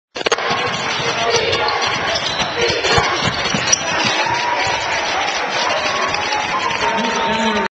Nick Emery of Lone Peak High
Nick Emery hitting a floater. 11-21-09,